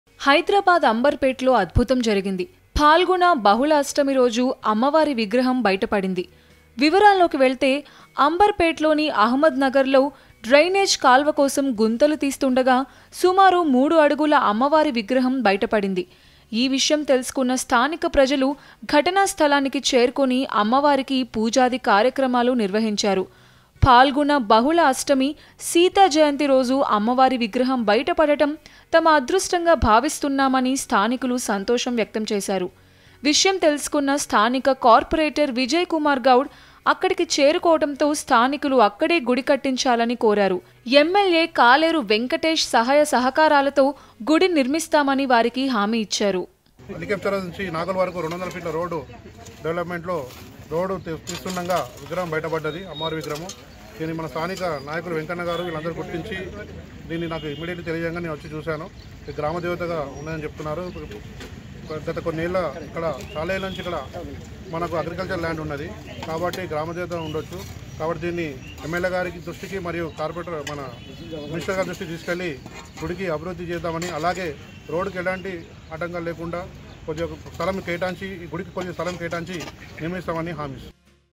అంబర్‌పేట్‌ తవ్వకాల్లో బయటపడ్డ అమ్మవారి విగ్రహం..భక్తులు ప్రత్యేక పూజలు -TV9
Credit: NewsUpdate | Rajeswari/Producer